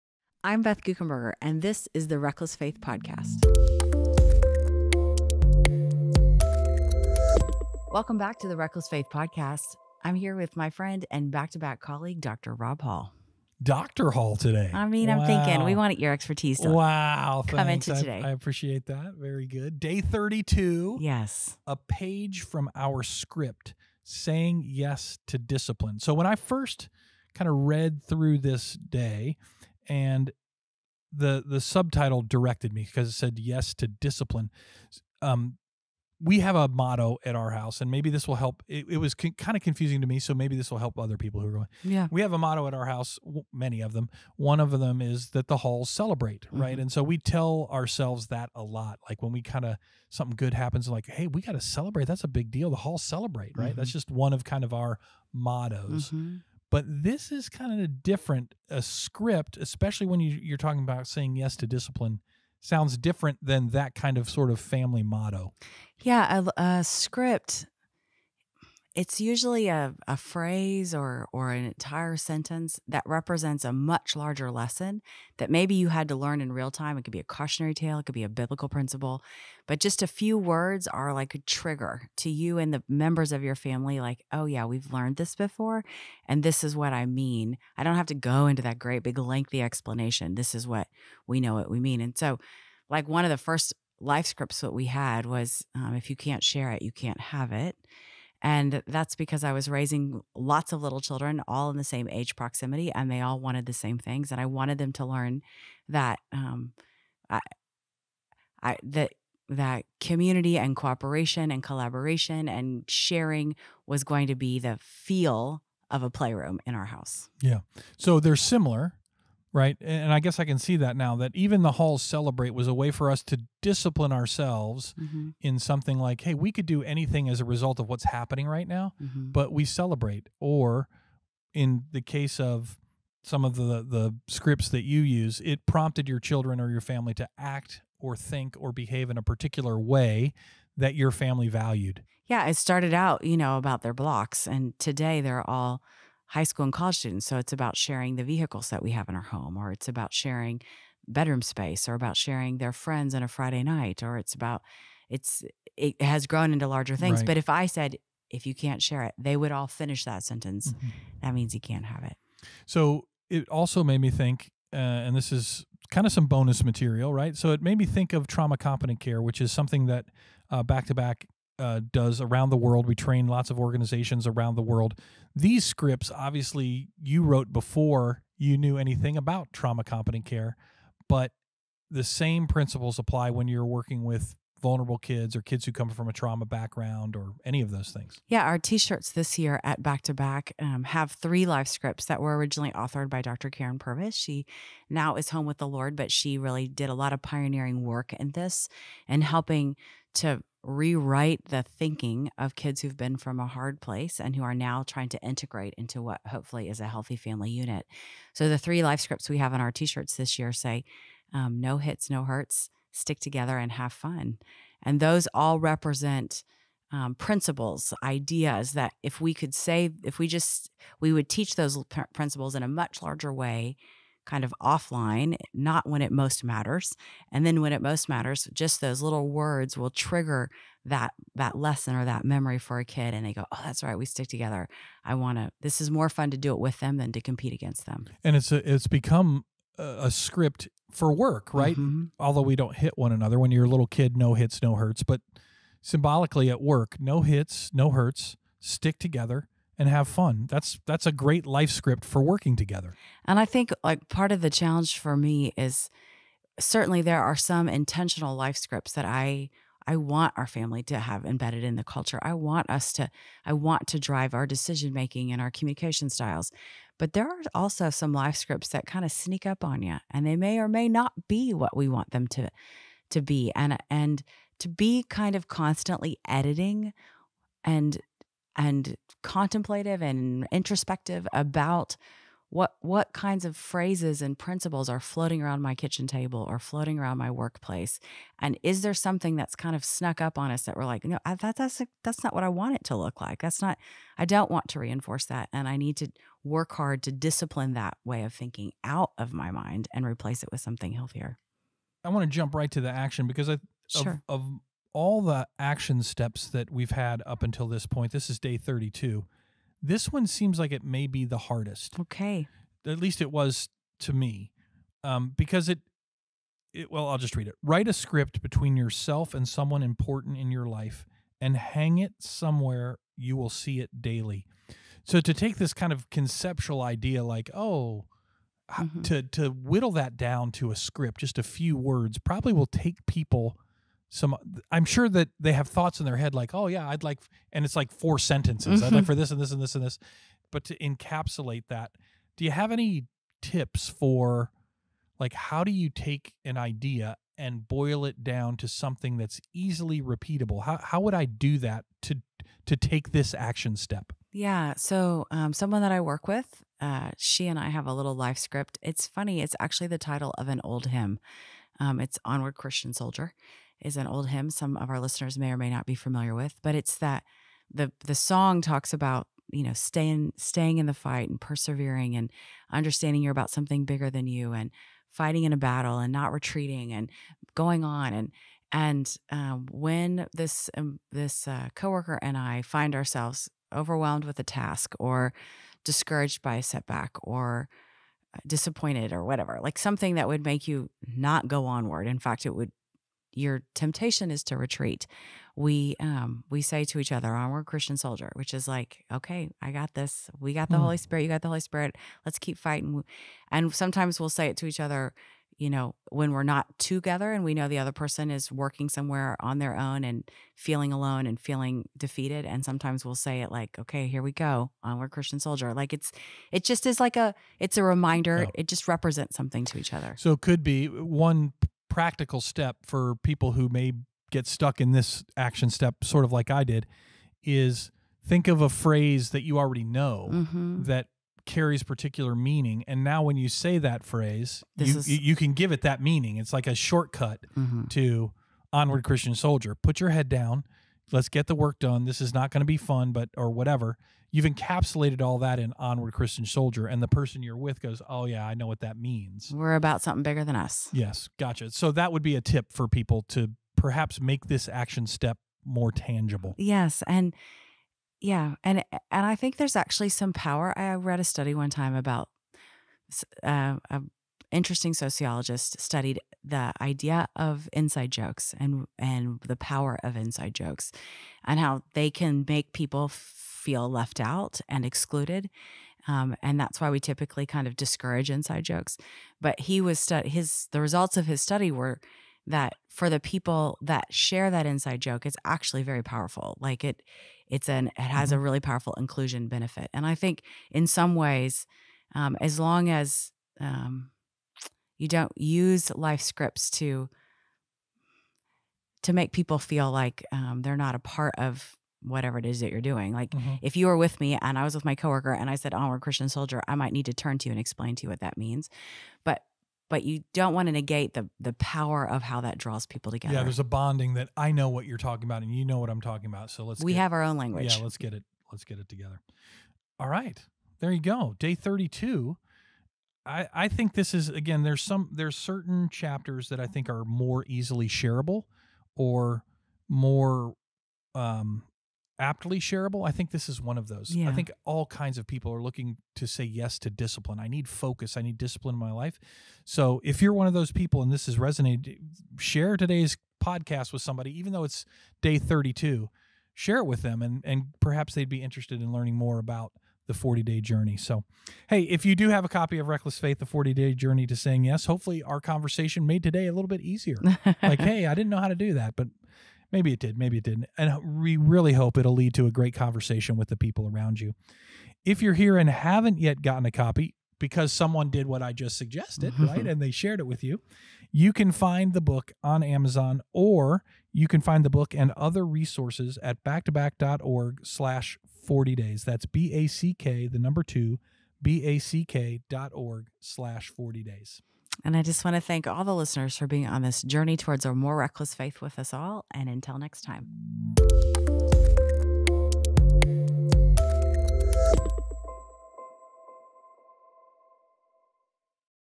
Episode 32 A Page From Our Script
Day thirty two: Say yes discipline. Beth and Rob discuss the difference between a "life script" and a motto. Beth shares some scripts she uses at home and at Back2Back Ministries, and how these life scripts have transformed and healed kids with traumatic backgrounds through Trauma Competent Care.